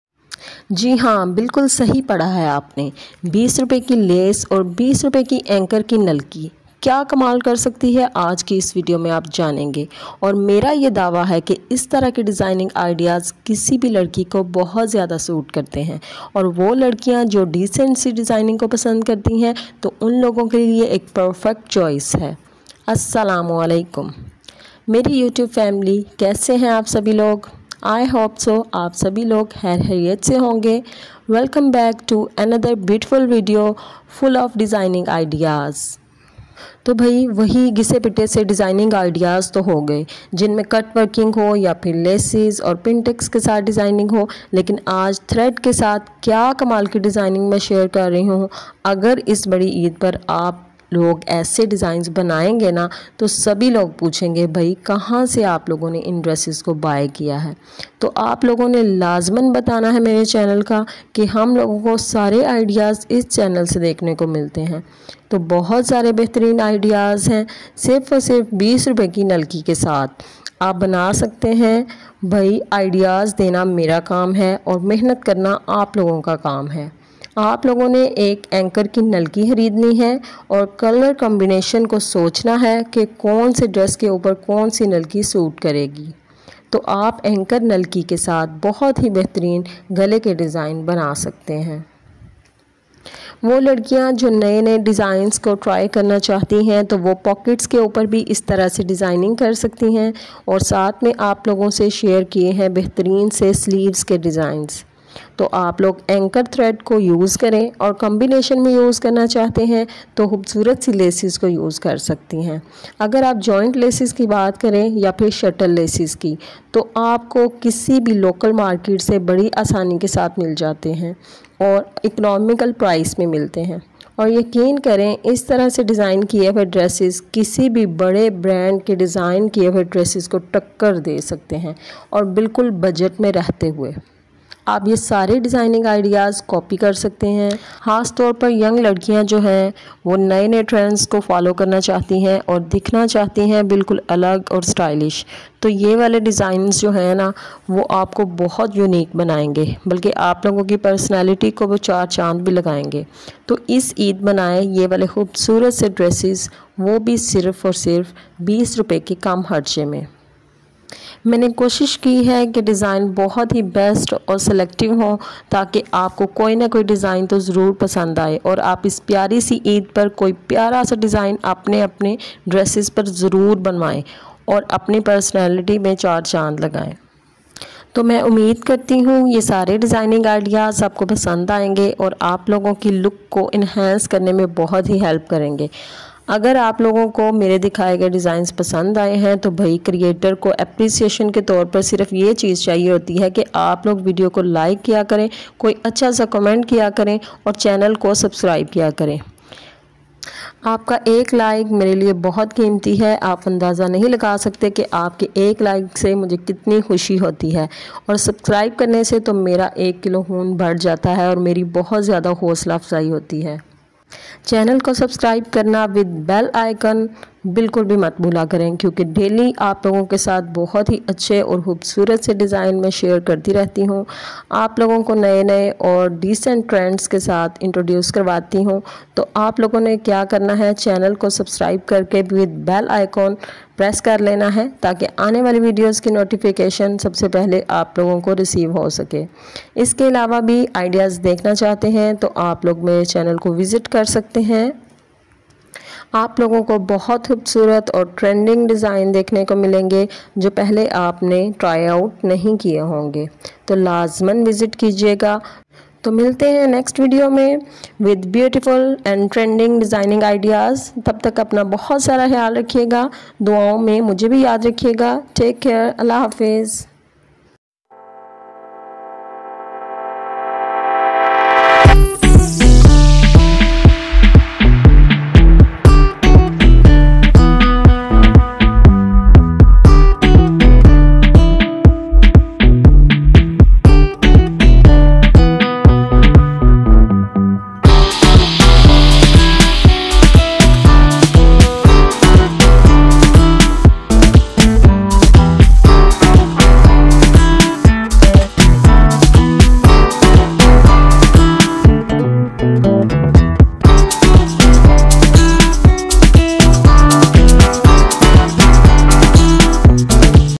20 Rs. Lace and Anchor Thread Dress Designs||Neck & Sleeves Design With Anchor Thread
This video is created to show neck and sleeves designs with anchor thread and lace designing ideas.I hope these designs will be helpful for you.